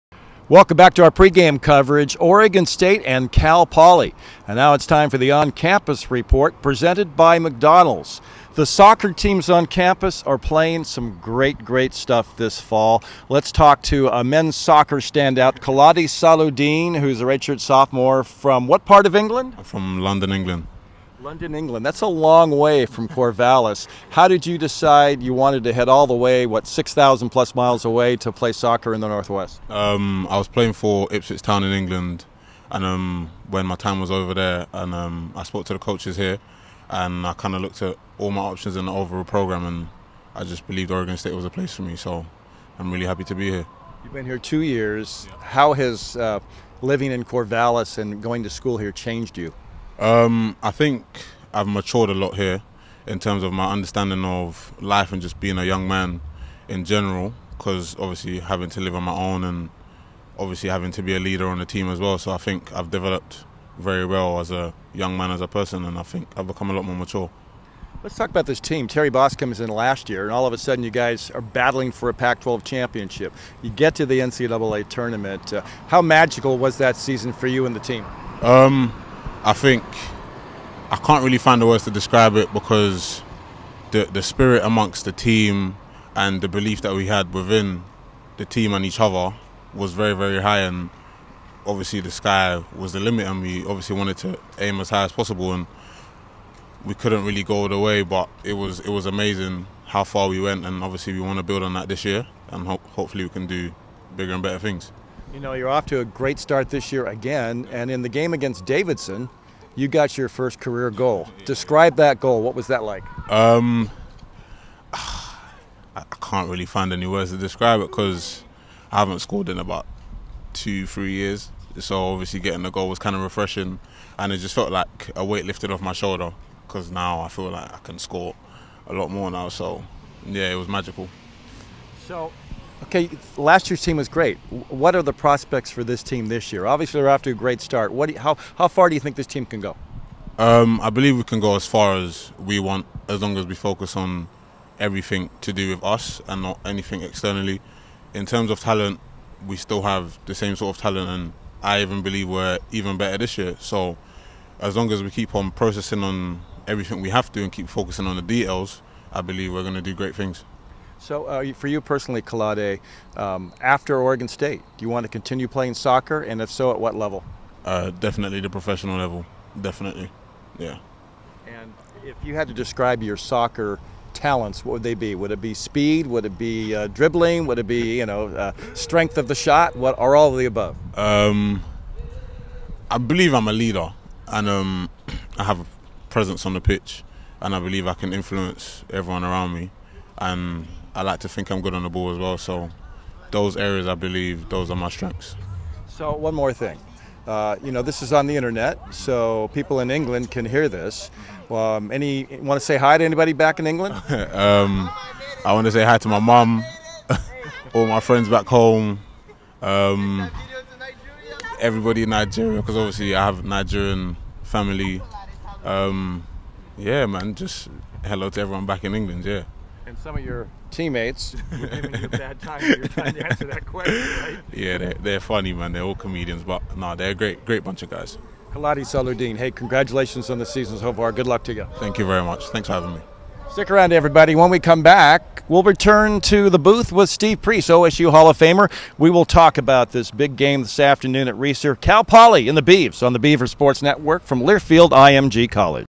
Kolade Salaudeen Oregon St
Men’s soccer standout talks about 2019 Beavs